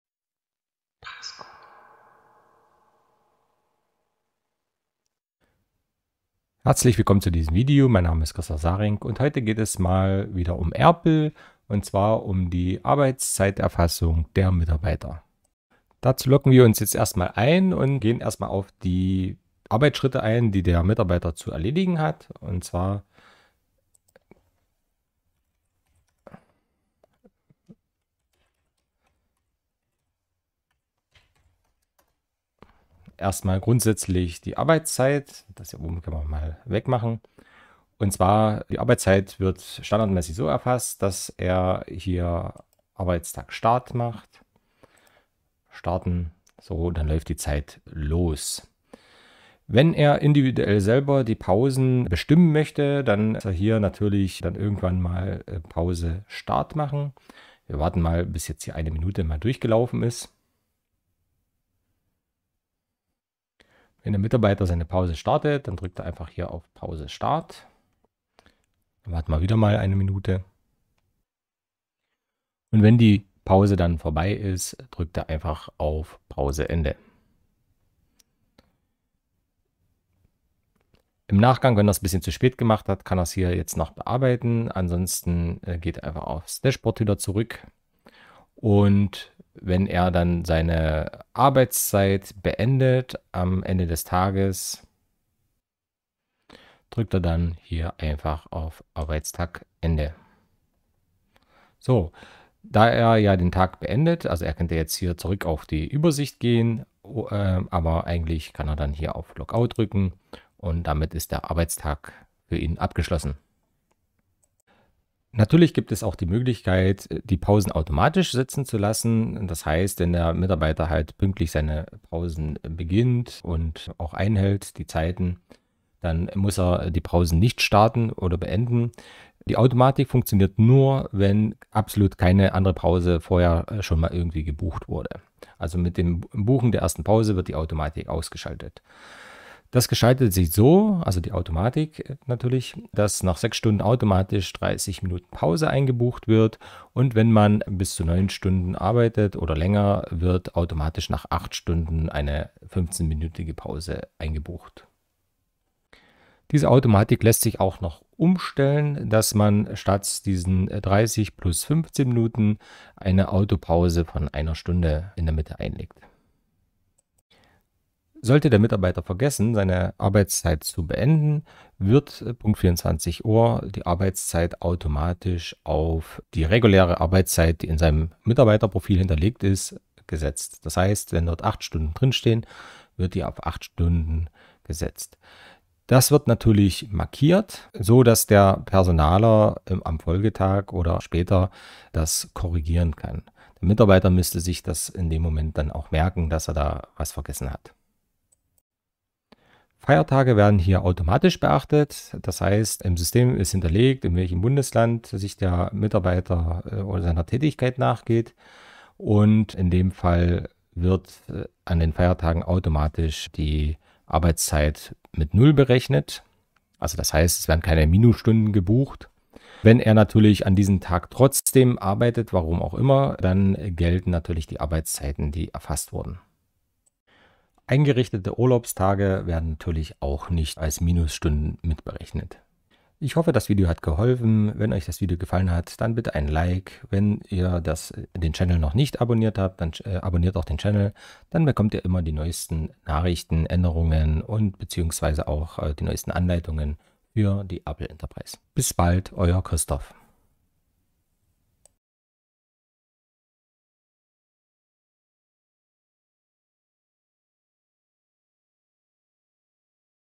ERPL Enteprise | Mitarbeiter Zeit Erfassung
Kleines Video für den Mitarbeiter ... Zeiterfassung mit ERPL Enteprise, lite oder Prosopiko, was ist zu tun und was funktionier wie?